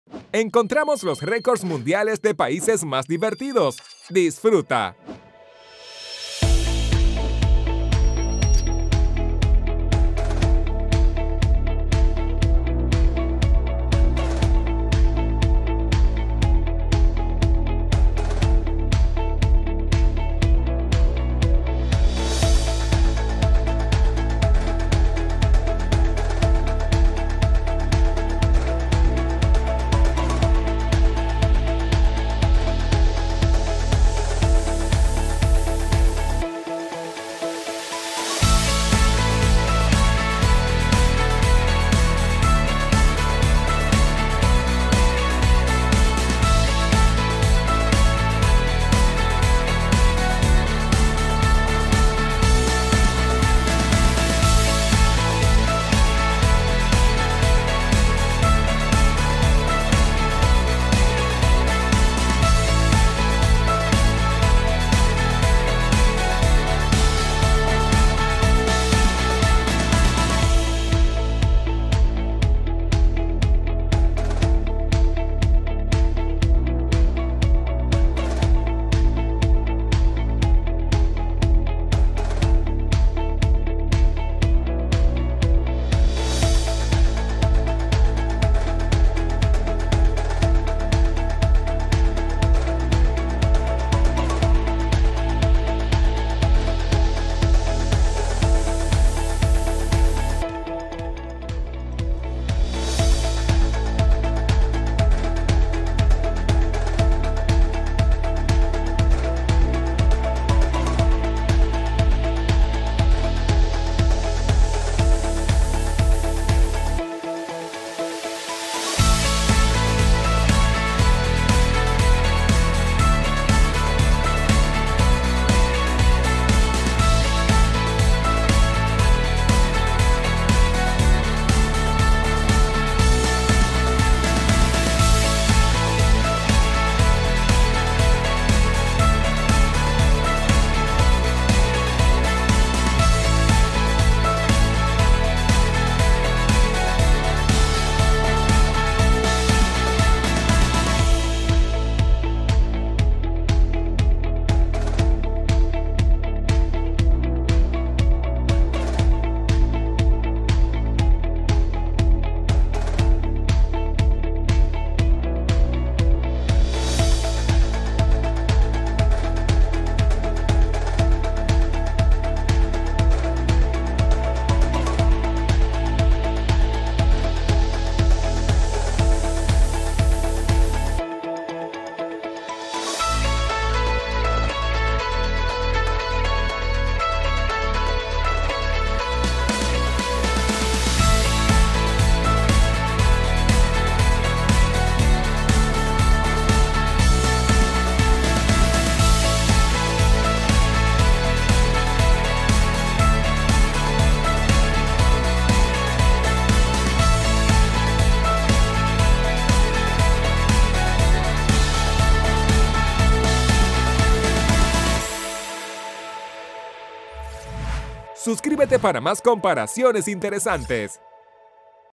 Comparación: Records Mundiales De Paises
Records de Paises, Desde Buenos a Malos! Que País Es Mas Inteligente? Cuál Tiene Mas Medallas Olimpicas o Equipo Militar? Cual es el Mas Poderoso? y el Mas Rico?

Desde el Pais Mas Chico al Mas Grande del Mundo. Incluímos los Records mas Interesantes que Encontramos!

Alguna vez te preguntaste cuál tiene mas población? o cuál es el país mas visitado? O que pais gano mas Medallas Olímpicas? Apostamos que desconocian algunos de estos.

Tambien rankeamos militarmente como cual tiene el mayor arsenal nuclear? O cuál tiene mas militares?

Tambien hay datos curiosos como. Cuál es el pais mas frio? O cuál es el mas cálido?

Tambien cubrimos temas como, cuál tiene mas IQ? O que país tiene mas expectativa de vida?
Que pais tiene la mayor deuda? Cuál es el pais mas Feliz? O Cuál es el pais mas Corrupto?

Algunos de los paises que aparecen en el video son Estados Unidos, Rusia, Reino Unido, China, Francia, Japón, Canada y muchos mas.

Que país sera el Top cuando hablamos del País con mas Millonarios?

Encontraras ambos, desde el país mas Seguro al mas Inseguro, Desde el mas Antiguo al mas Reciente, Que país tiene el pasaporte mas poderoso? País con mas consumo de alcohol? Paises con mas ratio de Crimenes?

Todas estas seran respondidas en este video! Si tu país no salió en este video es porque intentamos poner los records mas interesantes. Que les parece si hacemos un video de los records mundiales de Cada País?